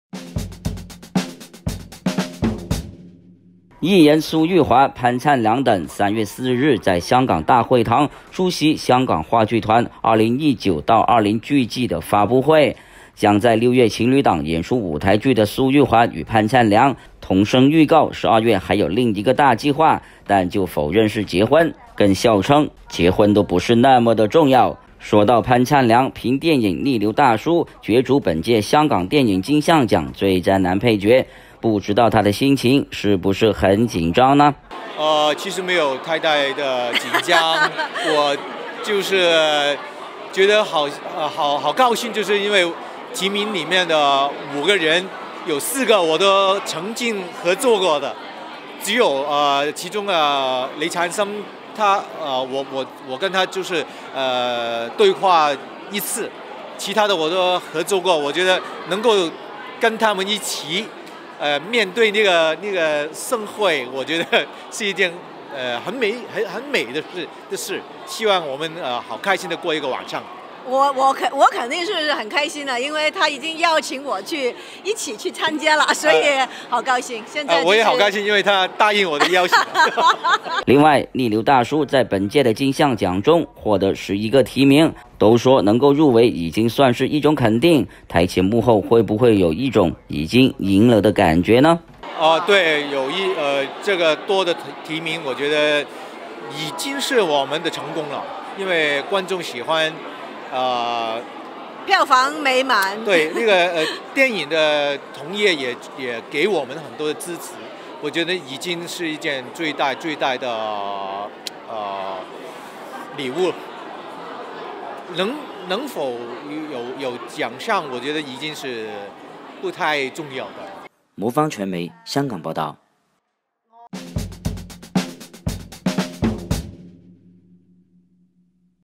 2019-03-04 潘燦良對爭奪金像獎男配角不緊張 蘇玉華：很開心他邀請我去
藝人蘇玉華 潘燦良等
3月4日在香港大會堂
出席香港話劇團2019-20劇季發佈會
將在6月情侶檔演出舞臺劇的蘇玉華與潘燦良
同聲預告12月還有另一個大計畫
但就否認是結婚
更笑稱結婚都不是那麼重要
說道潘燦良憑電影《逆流大叔》
角逐今屆香港電影金像獎最佳男配角
不知道他心情是不是很緊張呢